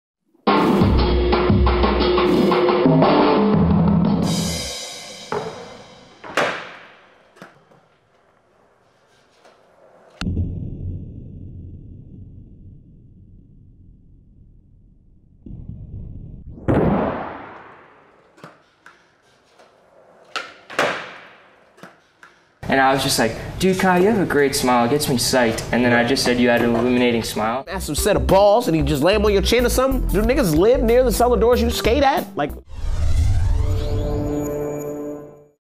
Chris Haslam Skateboarding in Slow Motion - Switch Frontside Back Foot Underflip
Watch your favorite skaters and tricks in Slow Motion.